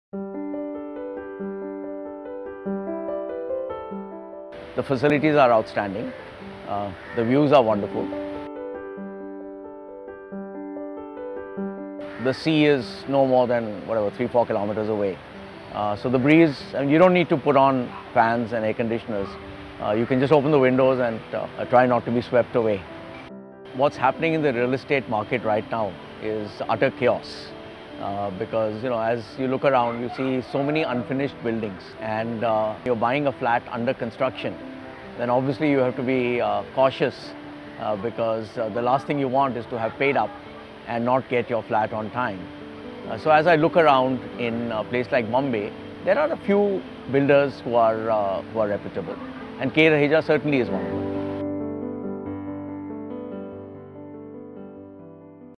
Mr. Sanjay Sakhuja found K Raheja Corp Homes reliable & honest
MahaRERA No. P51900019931

Where many people face hurdles with their unfinished dream
homes, Mr. Sanjay Sakhuja found K Raheja Corp Homes reliable &
honest with their on-time delivery.

Mr. Sanjay Sakhuja spoke about how impressed he is with the
facilities and thoroughly enjoying the beautiful sea view from his
balcony at Raheja Vivarea, Mahalaxmi.

We are glad to hear such kind words & will be servicing the
grandeur lifestyle that everyone deserves!

445 445.